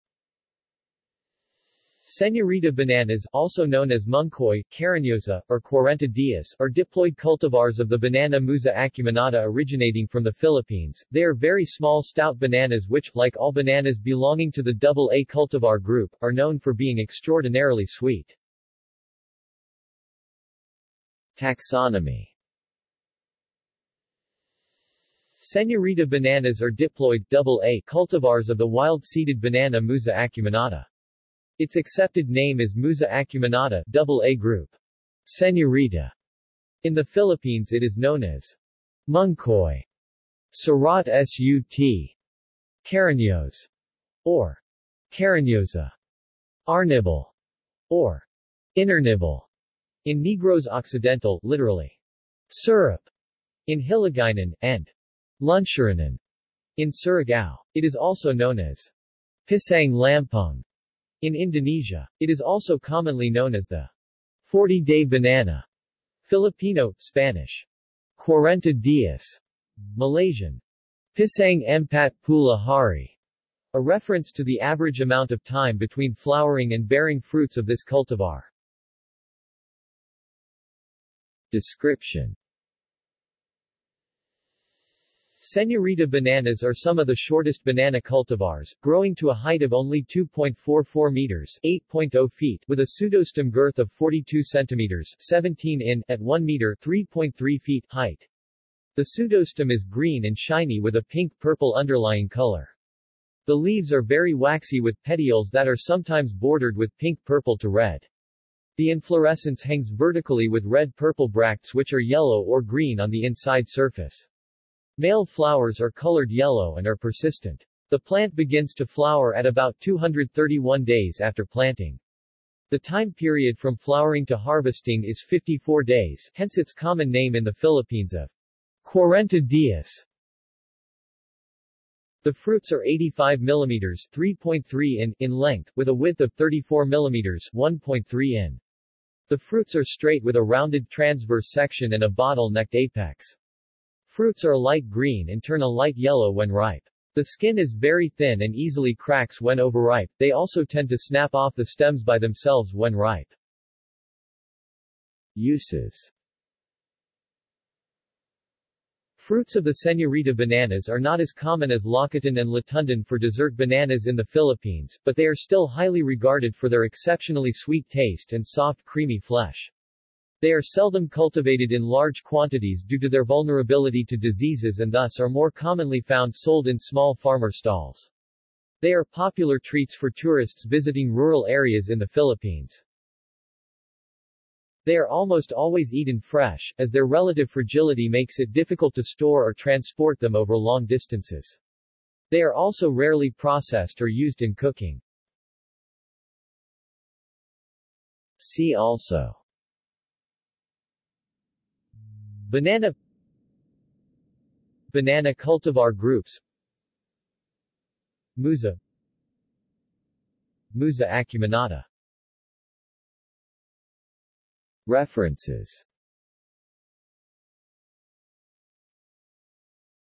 Señorita banana | Wikipedia audio article
This is an audio version of the Wikipedia Article:
Señorita banana

SUMMARY
Señorita bananas (also known as Monkoy, Cariñosa, or Cuarenta Dias) are diploid cultivars of the banana Musa acuminata originating from the Philippines.  They are very small stout bananas which, like all bananas belonging to the AA cultivar group, are known for being extraordinarily sweet.